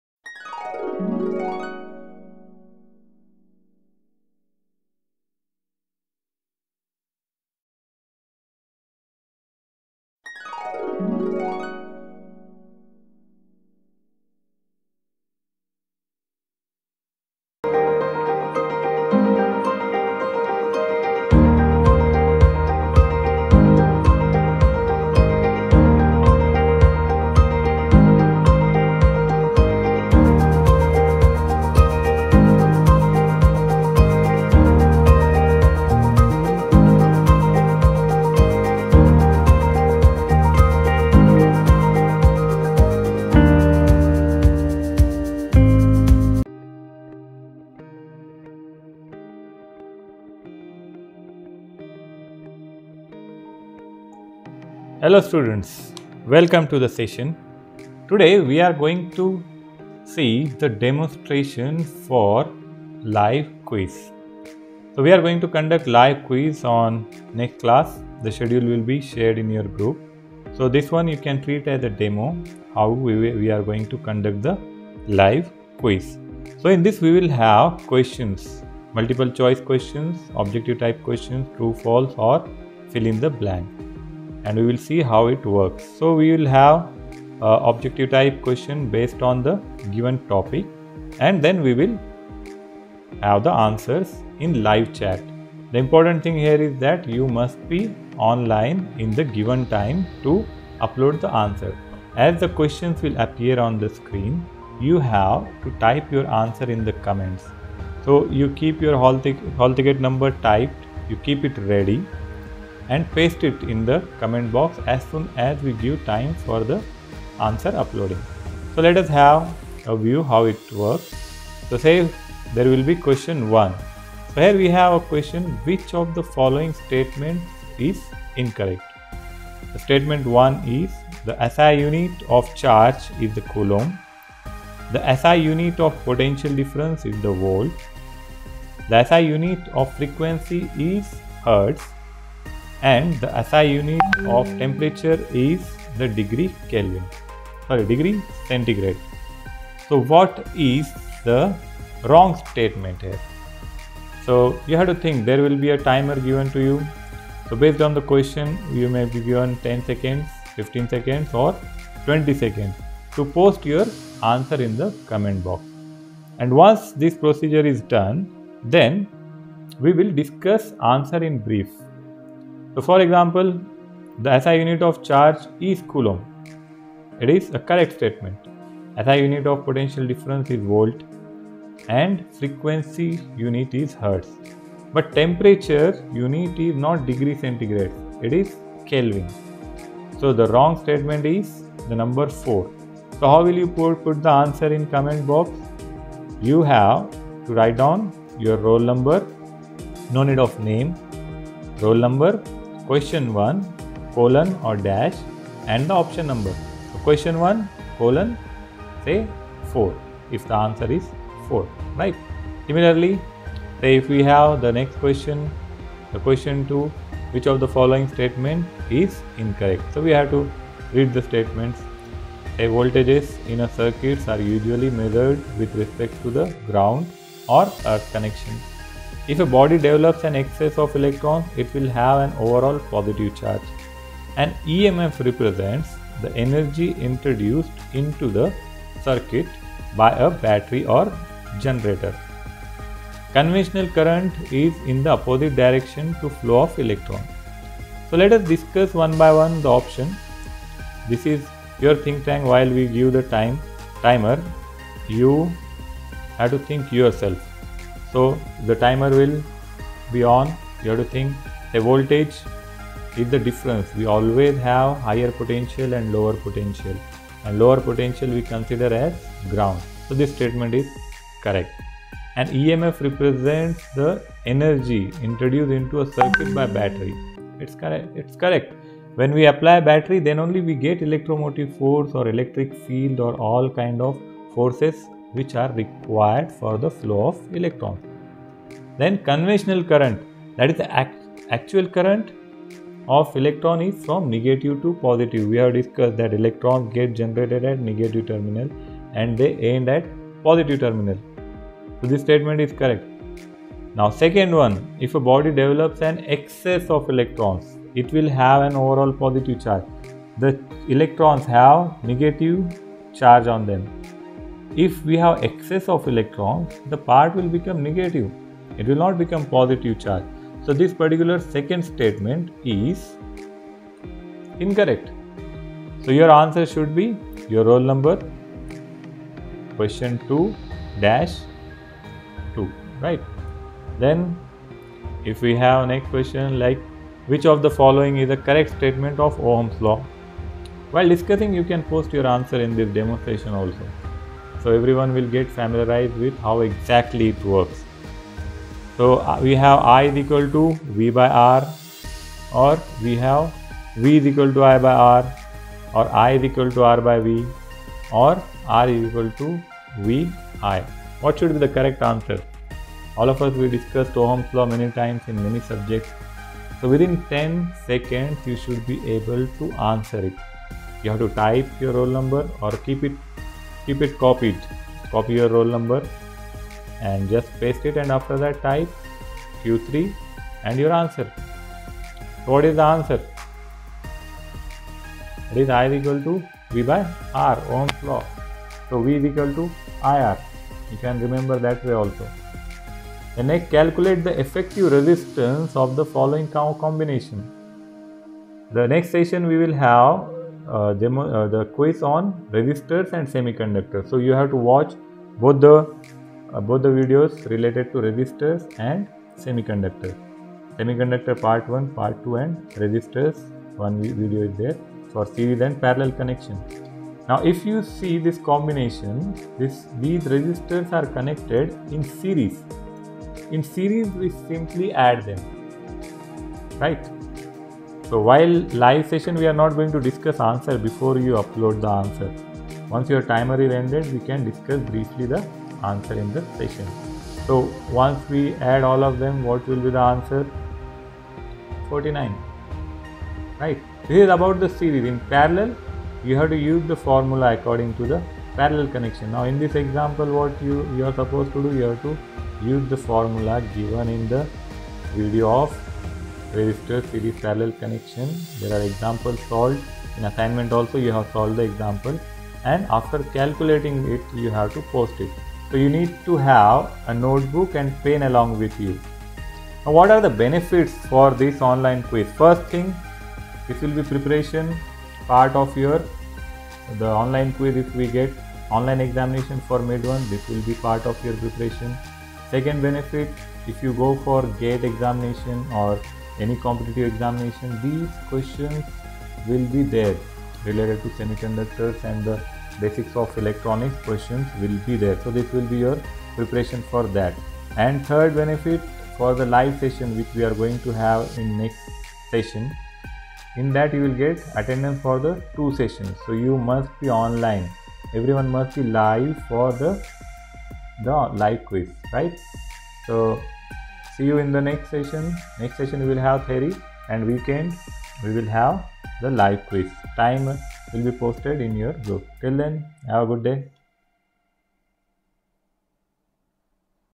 LIVE QUIZ DEMO : Topic: Resistors & Semiconductors
While watching the video post the answers in comment section of live chat.
Post your answers like this : Branch Name : Your Name, Question number , Answer number.
For example : CSE-A : Name :  Q1 : option 3
Time of the quiz : 6.15 PM ( 01/12/220)

WATCH COMPLETE VIDEO TO UNDERSTAND THE STEPS FOR UPCOMING LIVE QUIZ.